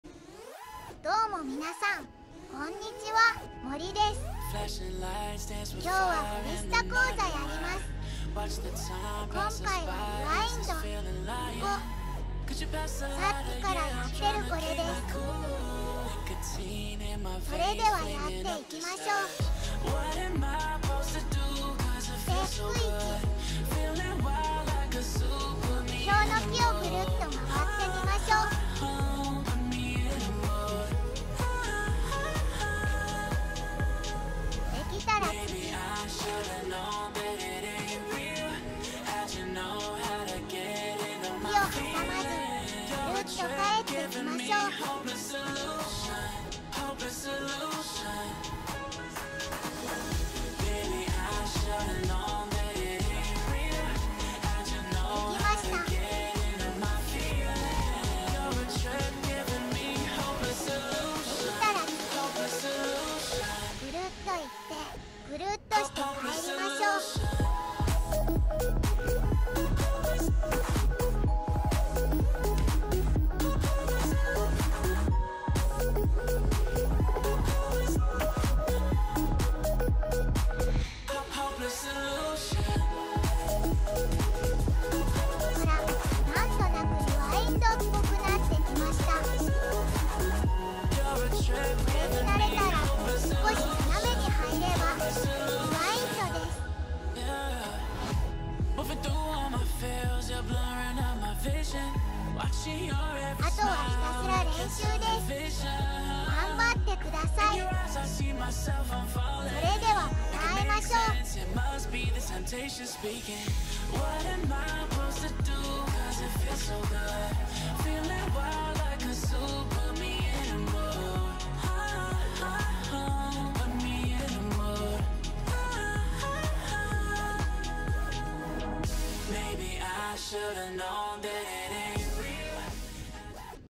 3分フリスタ講座！！VOL.2 「REWIND(横方向）」
3分でわかる！
フリースタイル講座！
その２です。
今回は希望の多かったリワインドです！